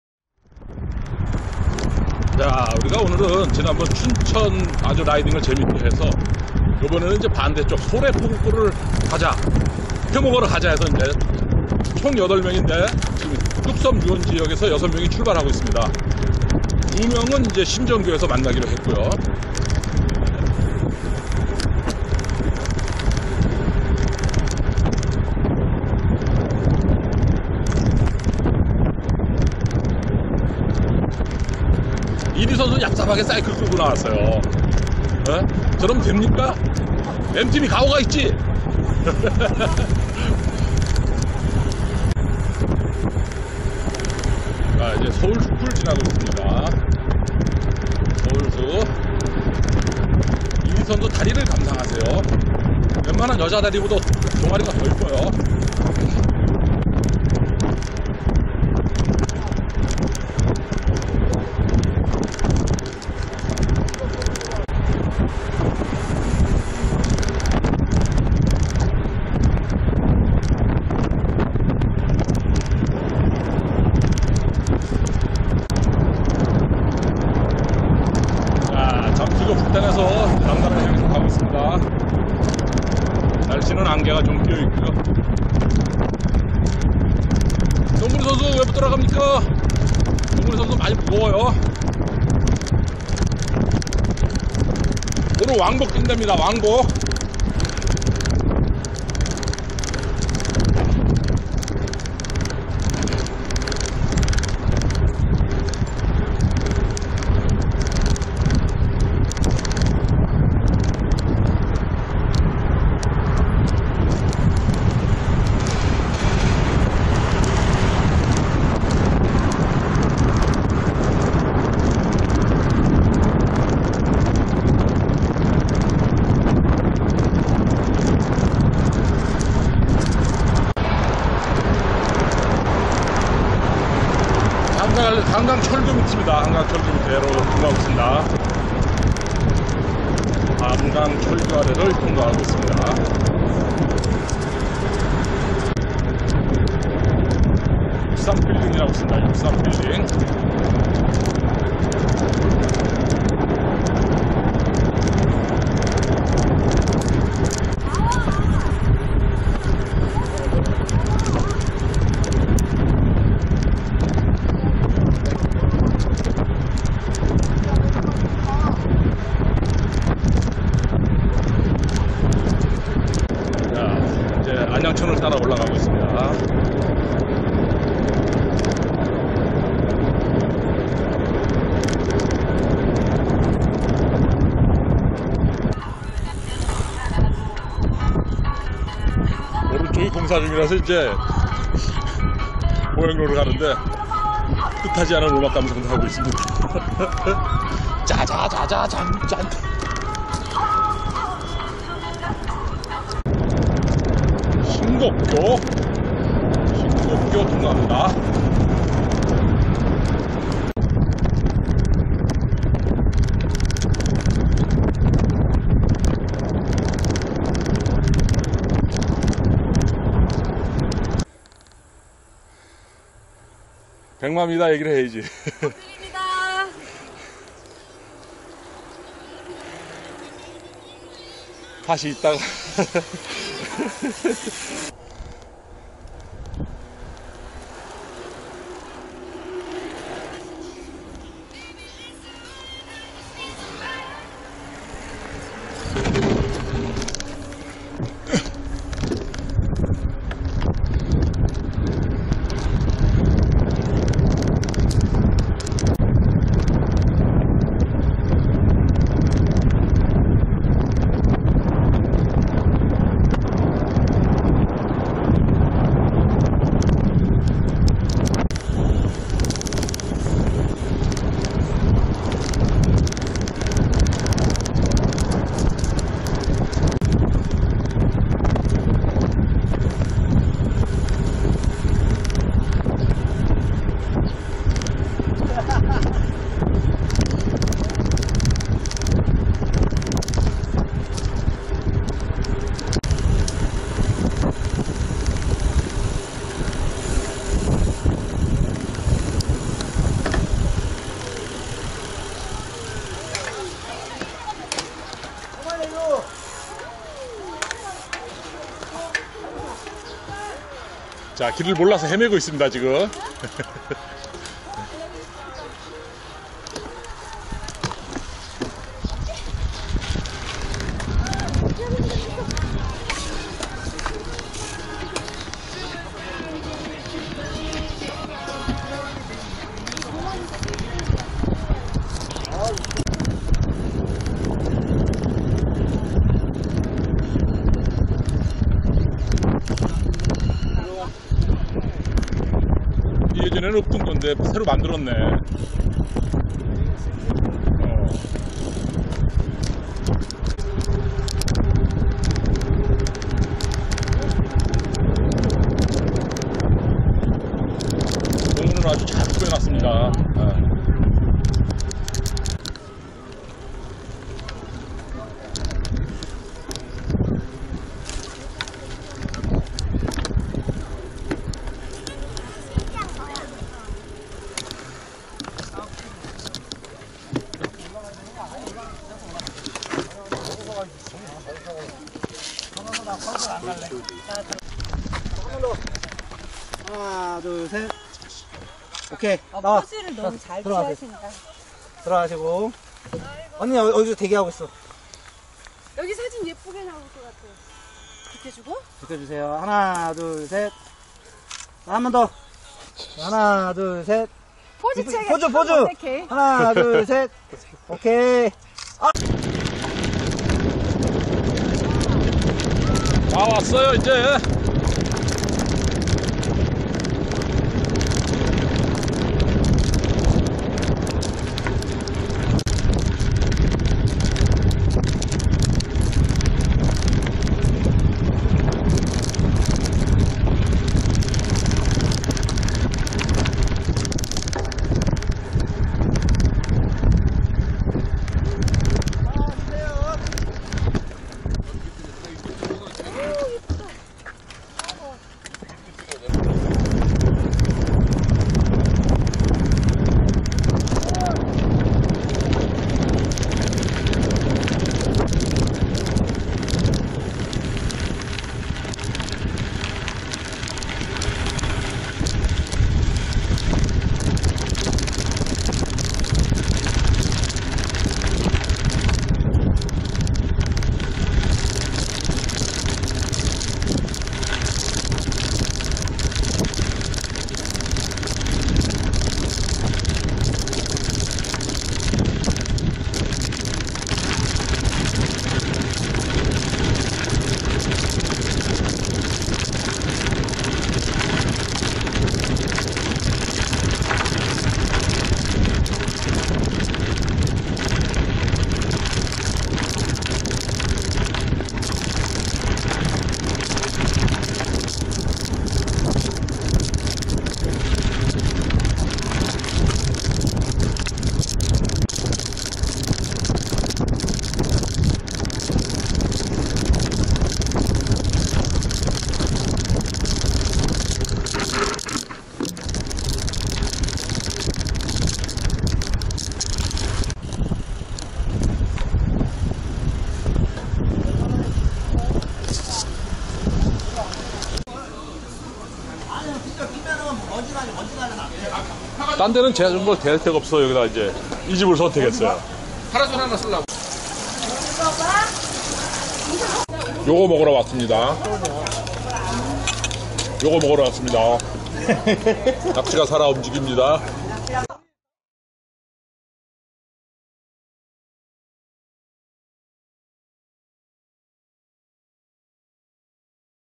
산산산MTB 춘천라이딩에 이어 소래포구 라이딩 포구에서 식사는 비추입니다
소래포구 식당등 바가지에 비위생적인거는 쫌 유명하지요?
회먹고와서 설사에 구토까지. 덴장~